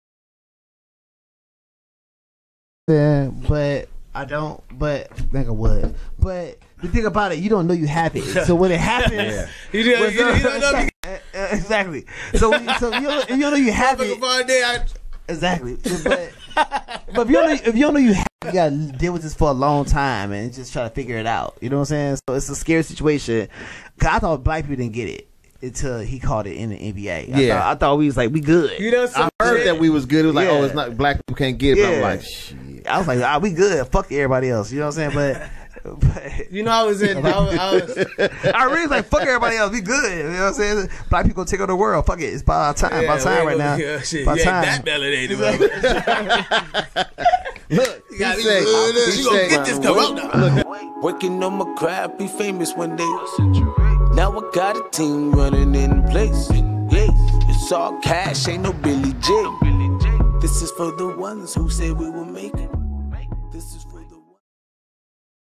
How Wrong He Was! Watch What Happens When He Meet A Black Person With The Rona.
Comedian & actor - Zo Johnson
 "I thought black people could not catch the virus, because of the melanin".
In this thought-provoking video, we explore a powerful encounter that challenges preconceived notions. Witness a candid conversation where one person's misconceptions about COVID-19 intersect with the experience of a Black individual.

Remember, it's important to use respectful and sensitive language when addressing sensitive topics like race and health. This revised description should help your video reach the right audience while maintaining a respectful tone.

Hosted by lead vocalist/drummer BIZ' the entertainer.

Powered by: @BeakonRadio
Streamed: Facebook.com/BeakonRadio